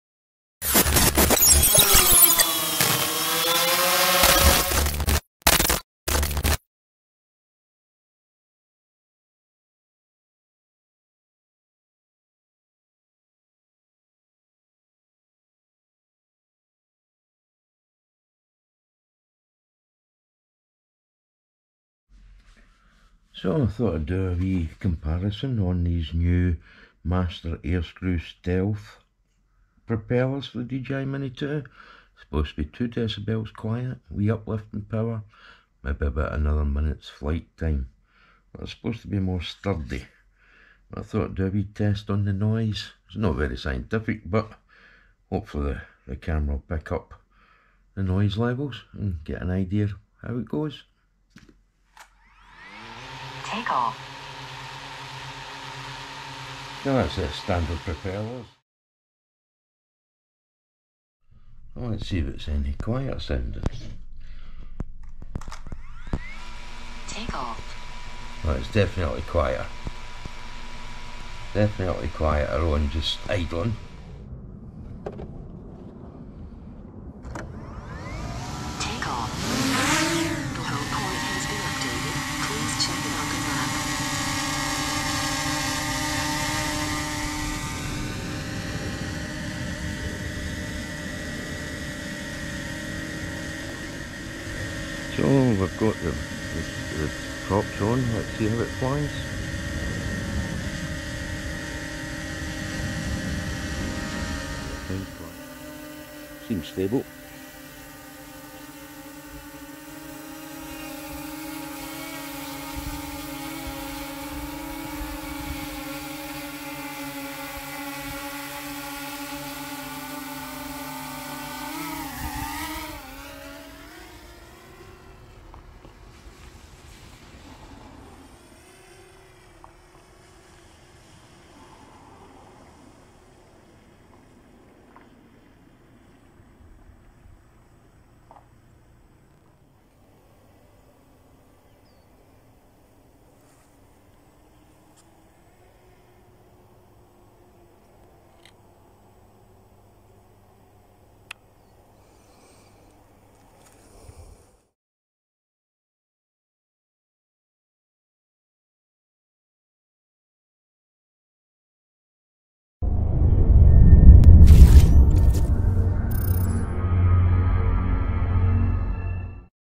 DJI MINI 2 MASTER AIRSCREW PROP UPGRADE
UPGRADING MY MINI 2 WITH THE MASTER AIRSCREW STEALTH PROPS